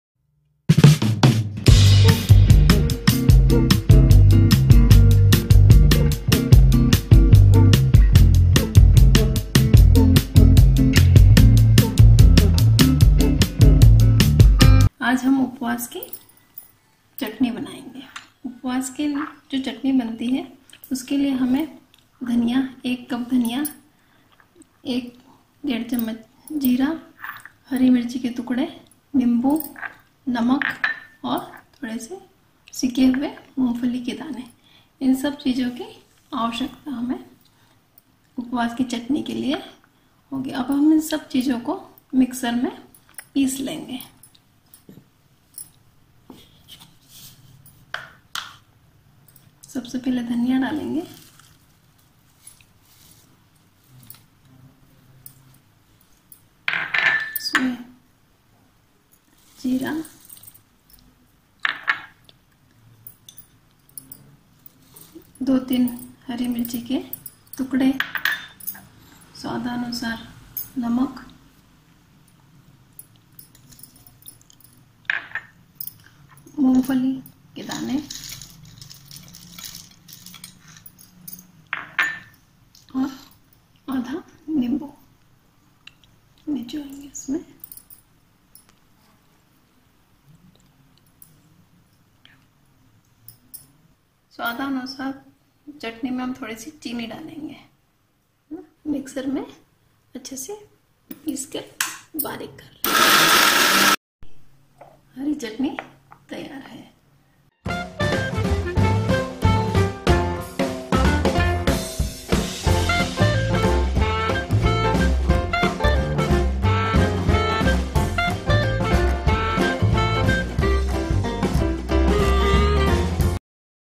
उपवास की फरियाली चटनी | गौरी लोमटे , सेंधवा
Upwaas fariyali green chutney |
 उपवास की फरियाली चटनी |